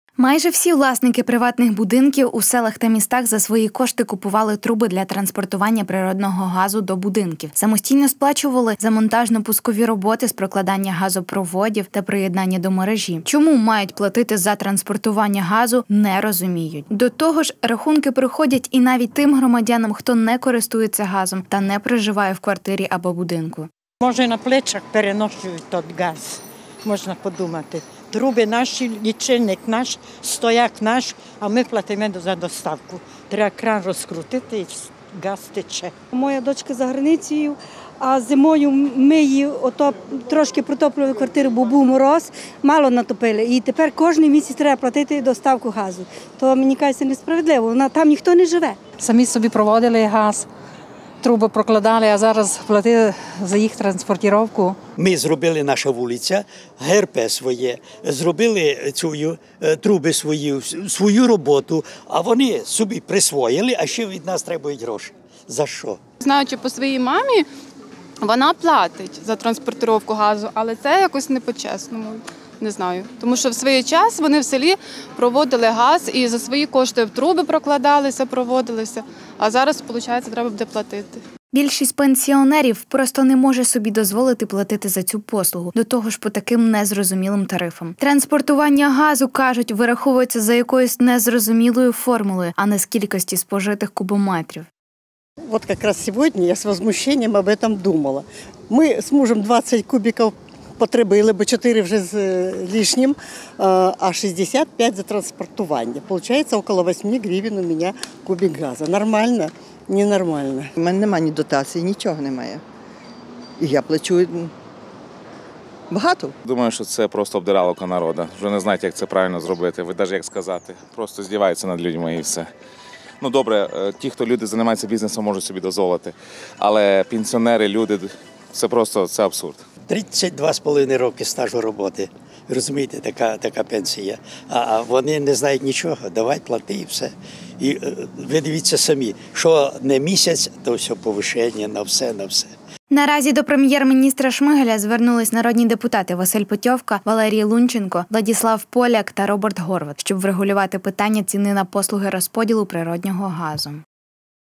Плата за транспортування газу
Майже всі власники приватних будинків у селах та містах за свої кошти купували труби для транспортування природного газу до будинків, самостійно сплачували за монтажно-пускові роботи з прокладання газопроводів та приєднання до мережі.  Чому  мають платити за транспортування газу – не розуміють.  До того ж,рахунки приходять і навіть тим громадянам, хто не користується газом та не проживає в квартирі або будинку. Більшість пенсіонерів просто не може собі дозволити платити за цю послугу,до того ж по таким незрозумілим тарифам.